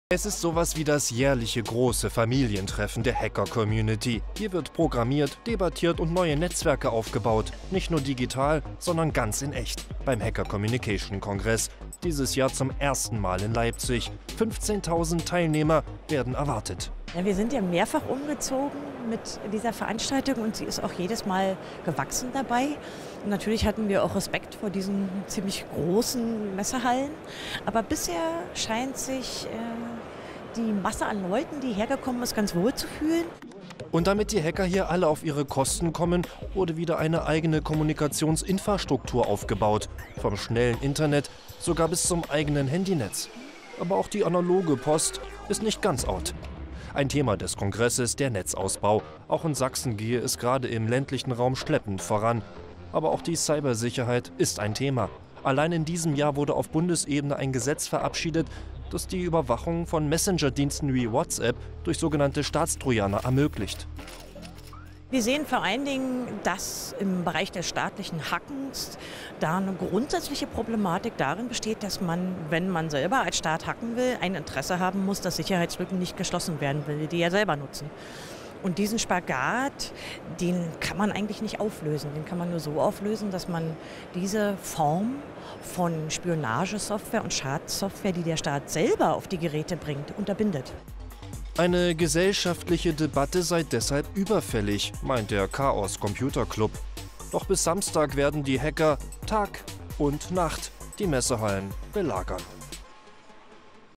MDR - Der 34c3 das Hacker-Familientreffen - Der Chaos Computer Club tagt in Leipzig - 27.12.2017
Ein Computer-Hacker ist nicht per se kriminell. Ein Teil der Szene will aufklären und vor Missbrauch schützen. Der Chaos Computer Club etwa mischt sich auch netzpolitisch ein. Er tagt aktuell in Leipzig.